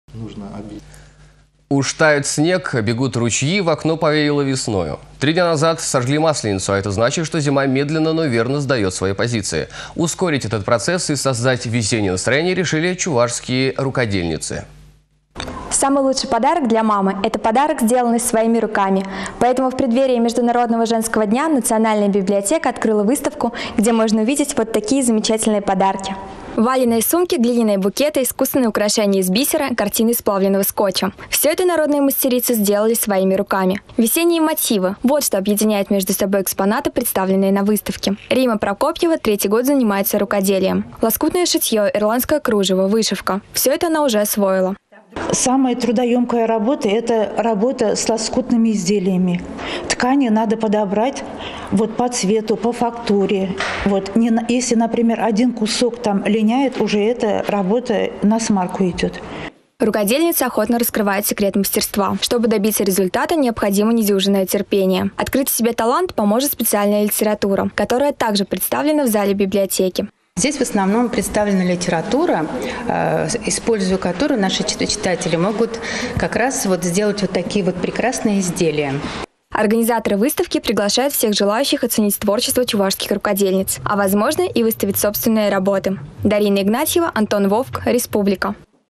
Весенние мотивы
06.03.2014
 «Уж тает снег, бегут ручьи, в окно повеяло весною». Три дня назад сожгли Масленицу, а это значит, что зима медленно, но верно сдаёт свои позиции. Ускорить этот процесс и создать весенние настроение попробовали чувашские рукодельницы.